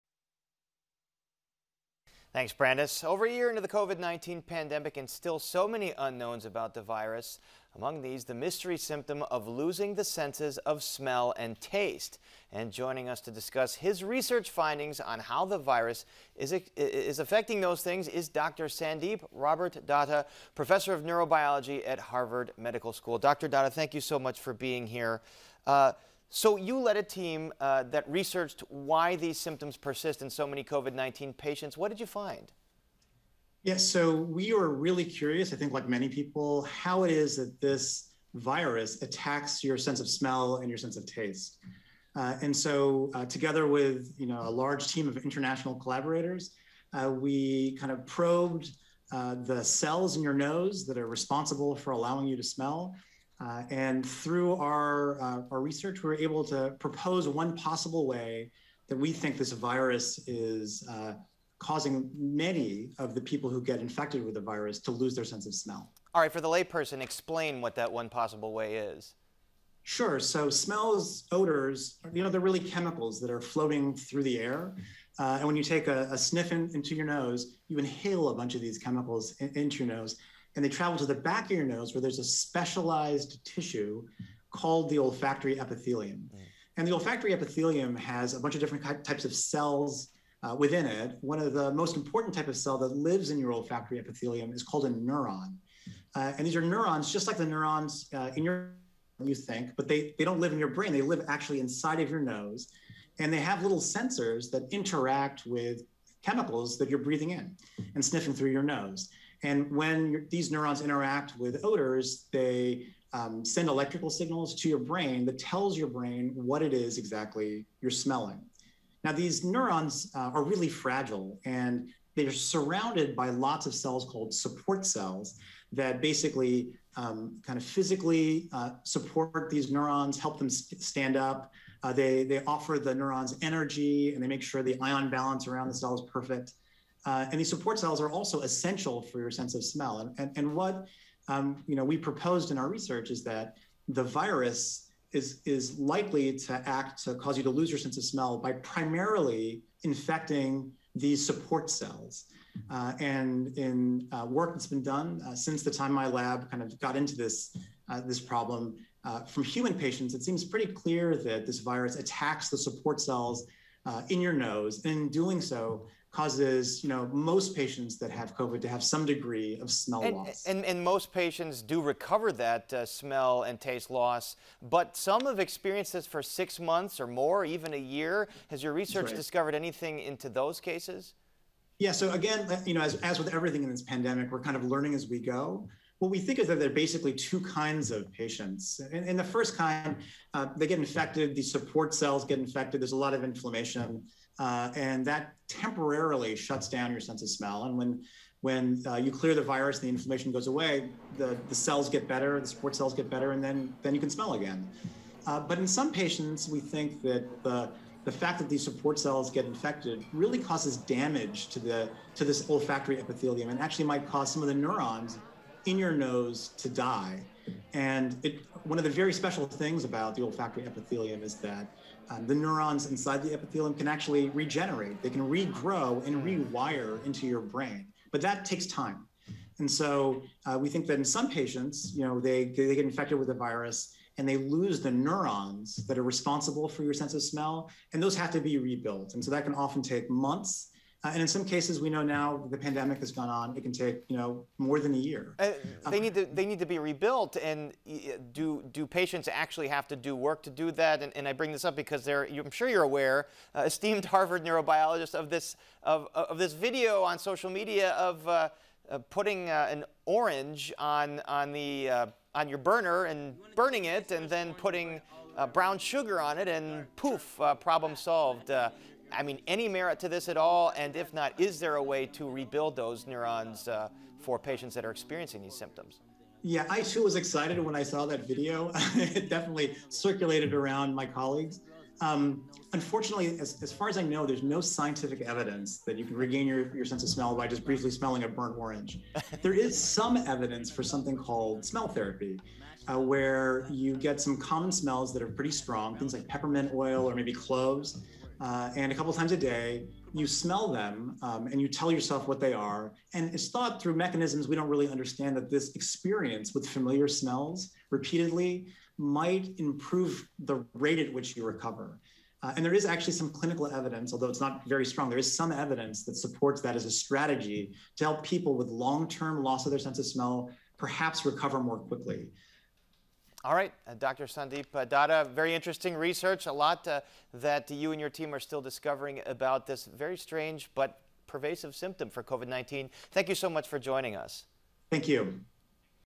New Research Explores How COVID-19 Affects Sense of Smell
There are still many unknowns surrounding COVID-19. Among these is the mysterious loss of taste and smell. Dr. Sandeep Robert Datta, a professor of neurobiology at Harvard Medical School, shares his recent findings.